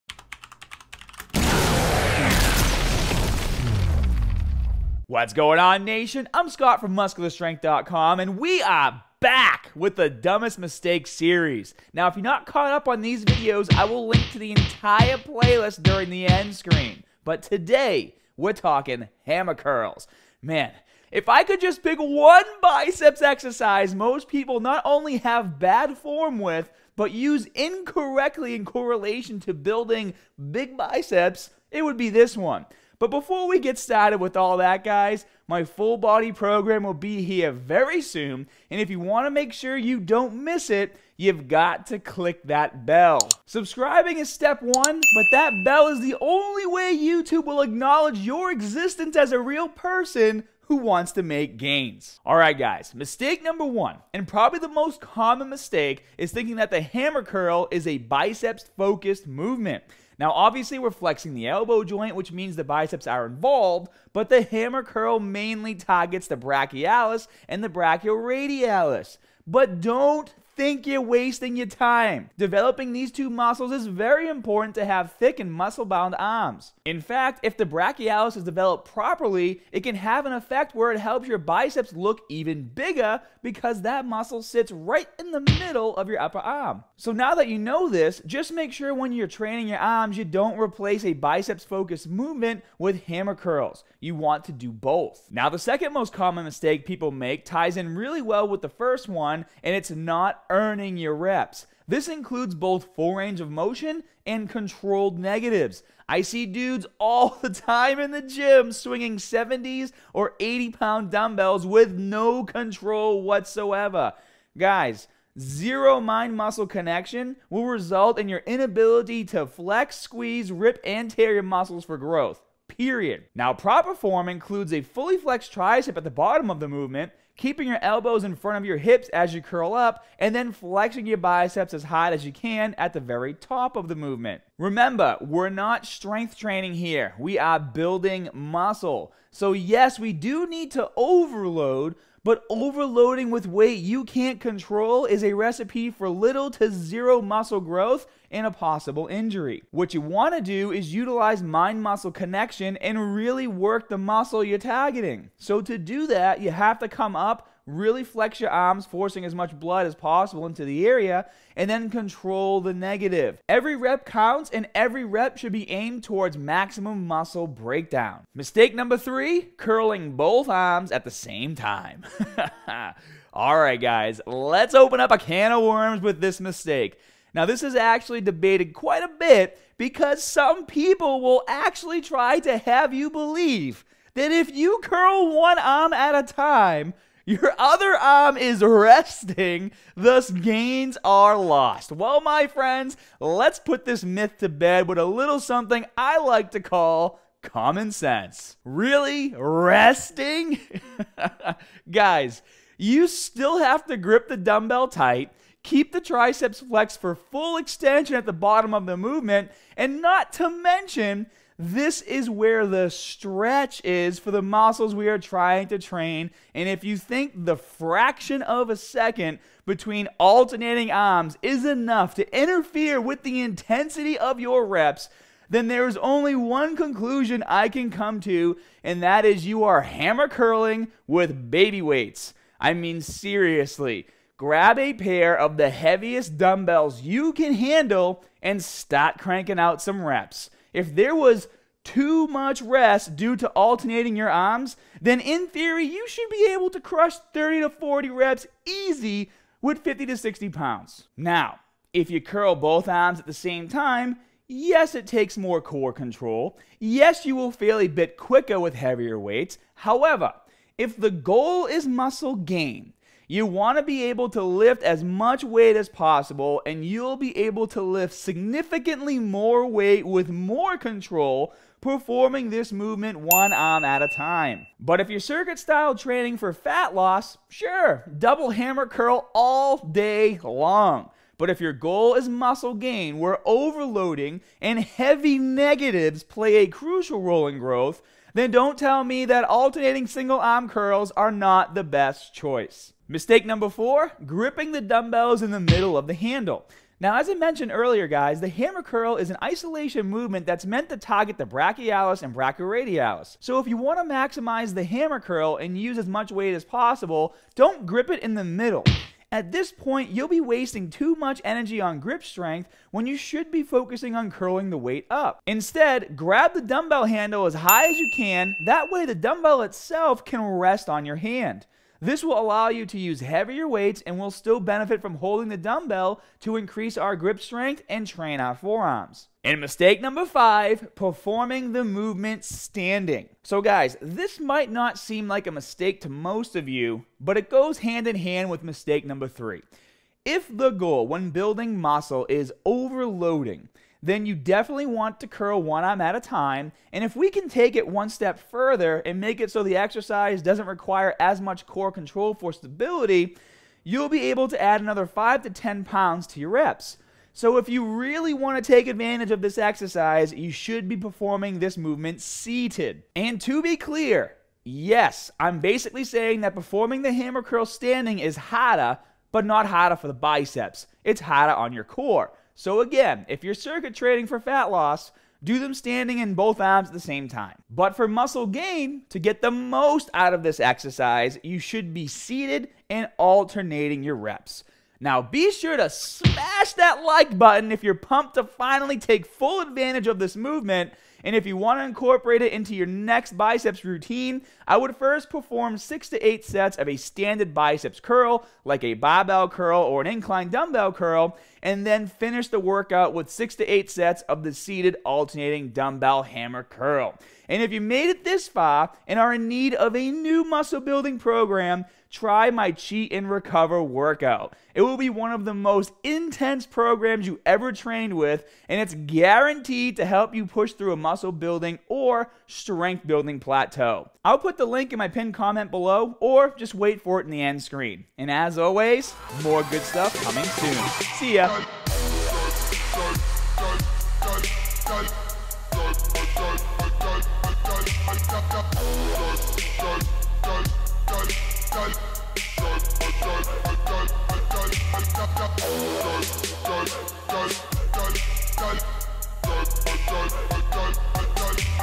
5 Dumbest Hammer Curl Mistakes Sabotaging Your ARM GROWTH! STOP DOING THESE!
Hammer Curls are a GREAT addition to any arm workout! BUT, they should never take the place of a standard curl whether using a barbell or dumbbells.  This is because the hammer curl is not a biceps focused movement!  It actually places more emphasis on the brachialis and the brachialradialis.  But that is just MISTAKE #1!  Tune in to make sure you are not making any more so you can MAXIMIZE YOUR GAINS!

It’s time for another installment in the dumbest mistake series! If you haven’t seen the other videos in this series, you can check out the full playlist HERE. Today, we’re talking hammer curls! If I could just pick one biceps exercise most people not only have bad form with, but use incorrectly in correlation to building big biceps, it would be this one.

Mistake #1: Treating The Hammer Curl As A Bicep Curl
This is probably the most common mistake. Obviously you’re flexing the elbow joint which means the biceps are involved, but the hammer curl mainly targets the brachialis and the brachioradialis. But don’t think you’re wasting your time! Developing these two muscles is very important to having thick and muscle-bound arms. In fact, if the brachialis is developed properly, it can have an effect where it makes your biceps look even bigger because that muscle sits right in the middle of your upper arm. Now that you know this, just make sure when you’re training your arms that you don’t replace a biceps focused movement with hammer curls – you want to do both!

Mistake #2: Not Earning Your Reps
This includes both full range of motion and controlled negatives. I see dudes all the time in the gym swinging 70lb or 80lb dumbbells with no control whatsoever. Zero mind-muscle connection will result in your inability to flex, squeeze, rip and tear your muscle for growth… period!

Proper form includes a fully flexed tricep at the bottom of the movement, keeping your elbows in front of your hips as you curl up, and then flexing your biceps as hard as you can at the very top of the movement. Remember, you’re not strength training here, you’re building muscle. So yes you do need to overload, but overloading with weight you can’t control is a recipe for little to zero muscle growth and a possible injury. What you want to do is utilize mind-muscle connection and really work the muscle you’re targeting. So to do that, you have to come up and really flex your arm to force as much blood into the area as possible and then control the negative. Every rep counts and every rep should be aimed towards maximum muscle breakdown.

Mistake #3: Curling Both Arms At The Same Time!
Let’s open up a can of worms with this mistake! This is actually debated quite a bit  because some people will try to have you believe that if you curl one arm at a time your other arm is resting, thus gains are lost. Well, let’s put this myth to bed with a little something I like to call common sense. Really, resting? You still have to grip the dumbbells tight and keep the triceps flexed for full extension at the bottom of the movement! Not mention, this is where the STRETCH is for the muscles you’re trying to train.

So if you think the fraction of a second between alternating arms is enough to interfere with the intensity of your reps, then there’s only one conclusion I can come to and that is that you are hammer curling with baby weights. Seriously, grab a pair of the heaviest dumbbells you can handle, and start cranking out some reps. If there was ‘too much rest’ due to alternating your arms, then in theory you should be able to crush 30 – 40 reps easy with 50lbs – 60lbs.

Now, if you curl both arms at the same time, yes it takes more core control, and yes you will fail a bit quicker with heavier weights. However, if the goal is muscle gain, you want to be able to lift as much as possible and you will be able to lift significantly more weight with more control performing this movement one arm at a time. But, if you're circuit-style training for fat loss, sure, double hammer curl all day long!  But if your goal is muscle gain where overloading and heavy negatives play a crucial role in growth, then don’t tell me that alternating single-arm curls are not the best choice.

Program Selector - Get The Right Program For You!